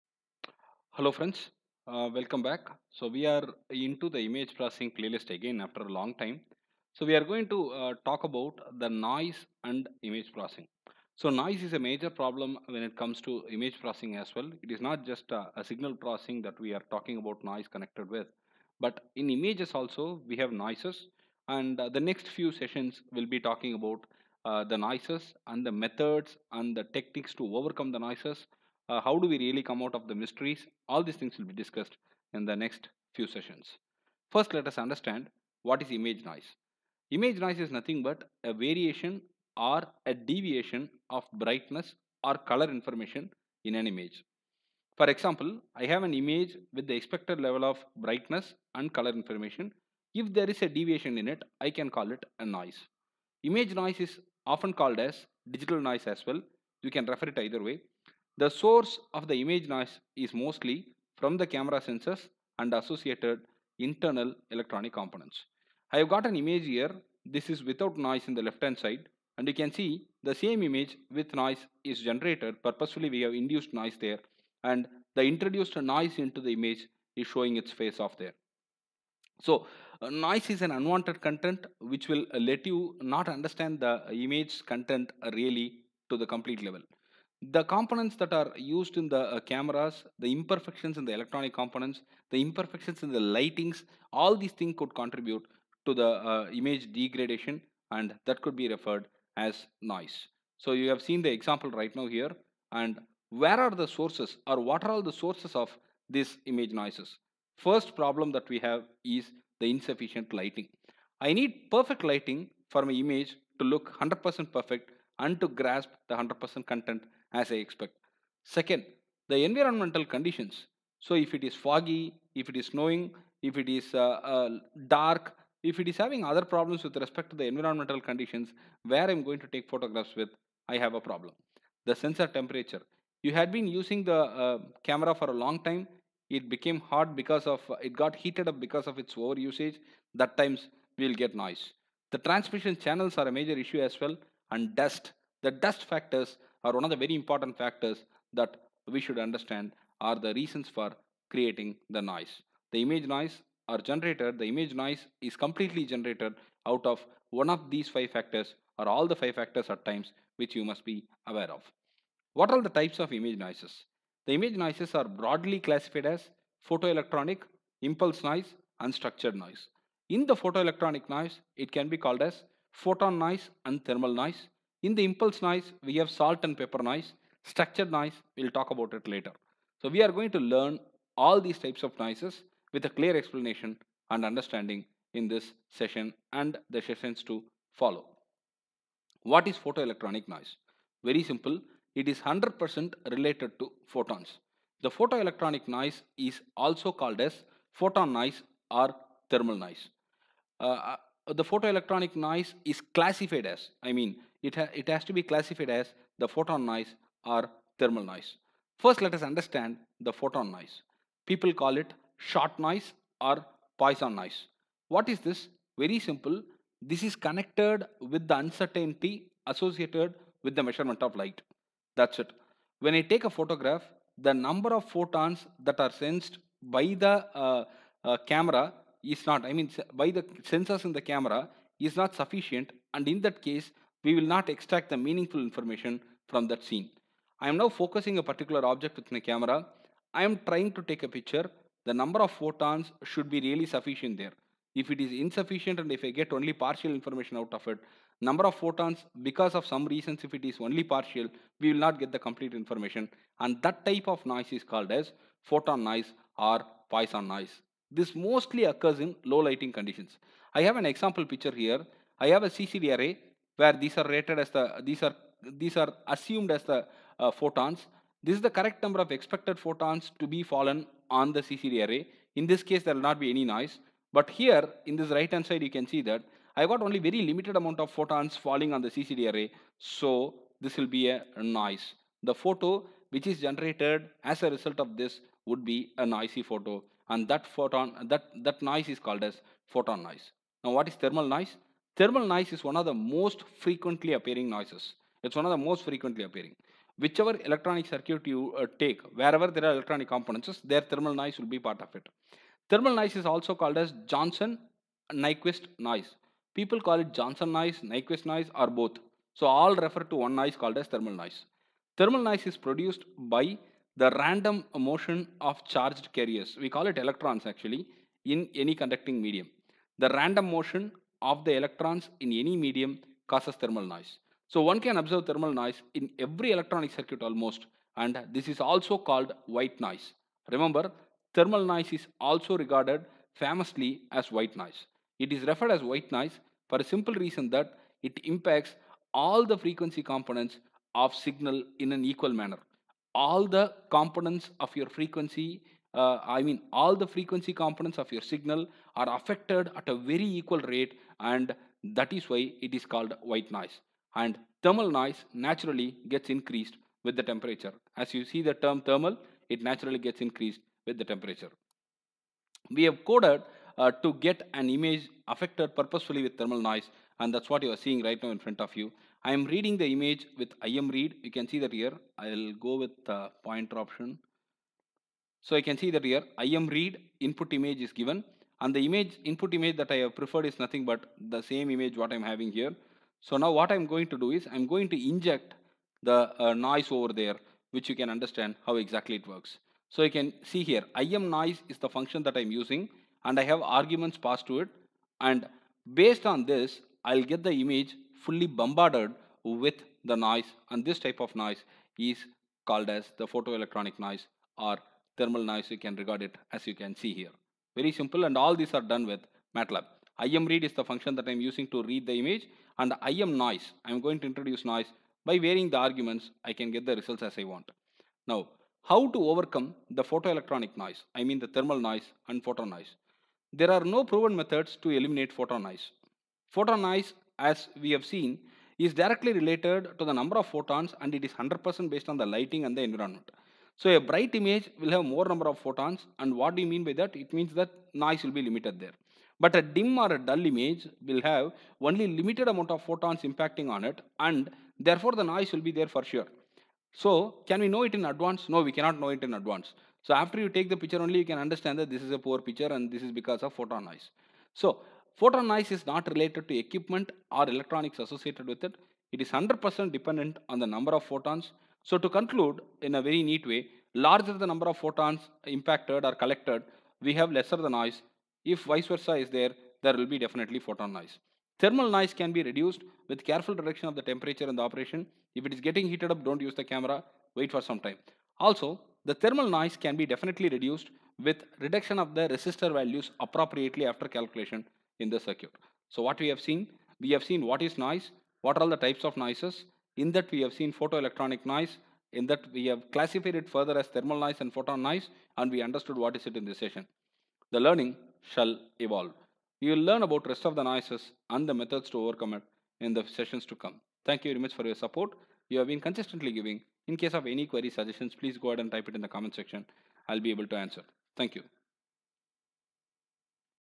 Session 14 - Noise in image processing
Here, in this session, we understand what Noise is all about in Image Processing.